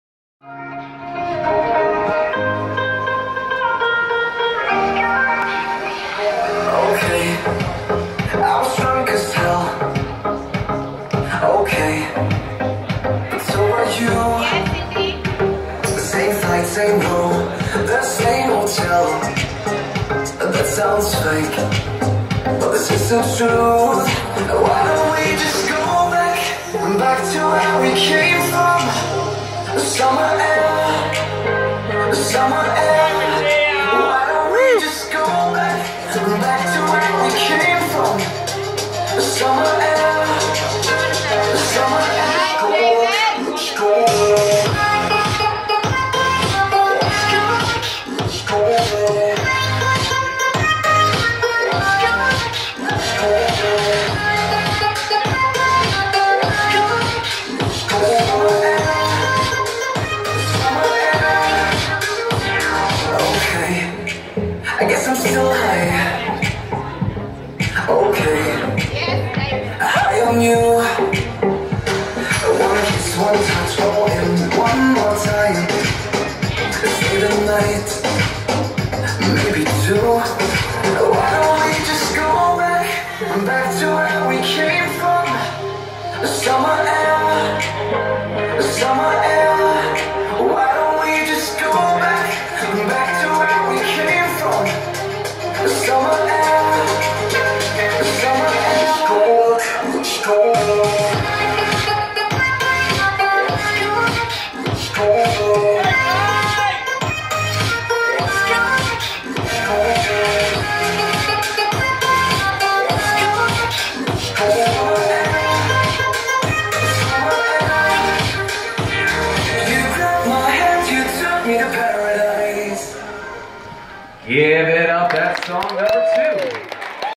Best of the Best 2017 - Novice Jack and Jill Pre-lims (Ty Randle and Ruth)
Best of the Best 2017 - Best of the Best 2017 - Novice Jack and Jill Pre-lims (Ty Randle and Ruth, David Phan and Matteah Ackers-Porter, Andrea Rindone and Georgia Craft, Sharon and Kel)